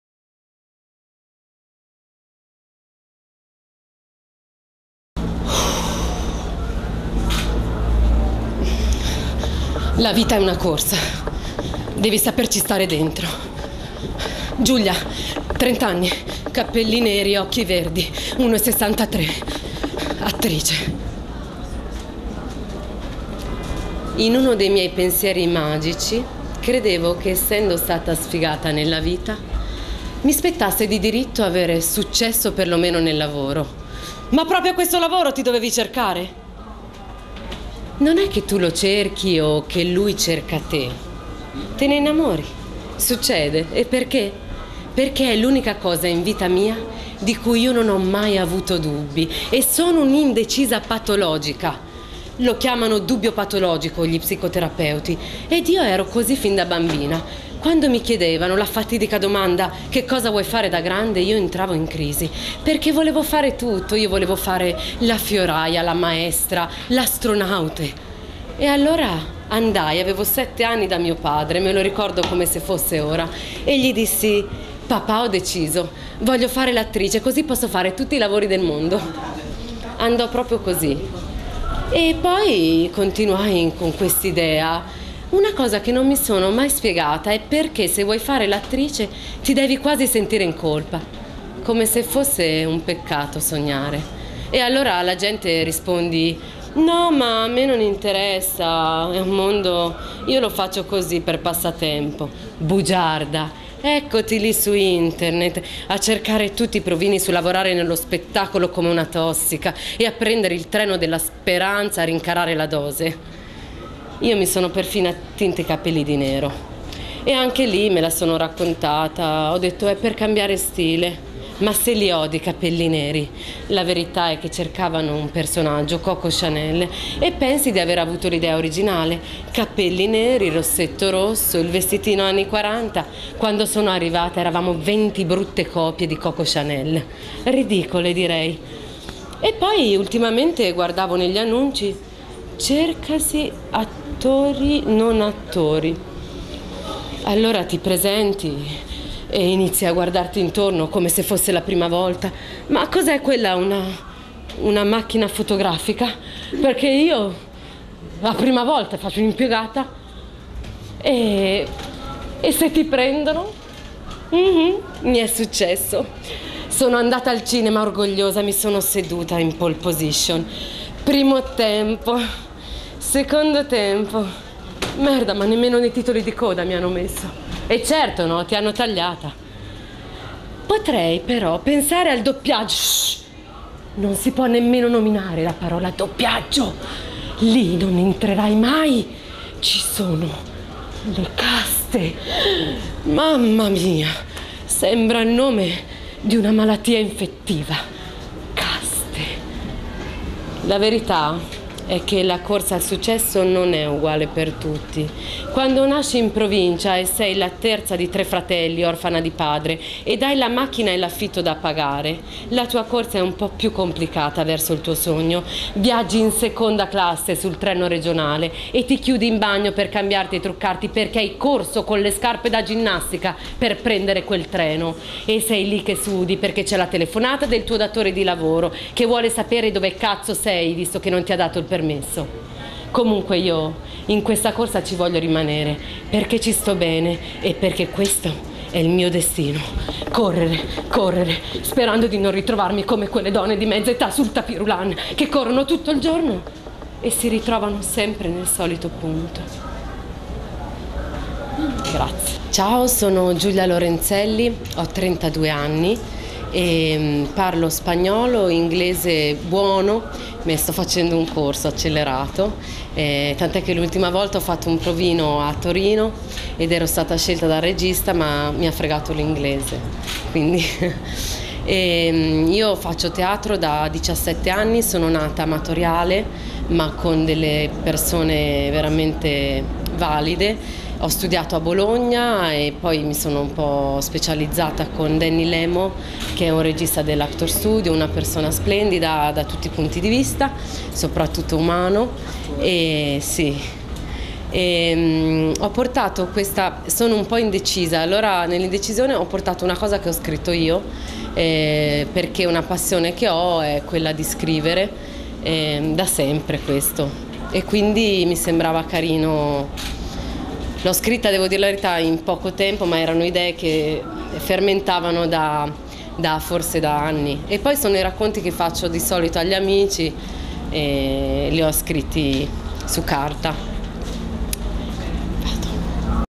Giulia Lorenzelli, Provino, 9 Giorni di Grandi Interpretazioni, 2012, Il Gioco Del Lotto, RB Casting
Giulia Lorenzelli, Provino, al Festival Internazionale del Film di Roma, Nove Giorni di Grandi Interpretazioni, 2012, promosso da Il Gioco Del Lotto, e Presentato da RB Casting,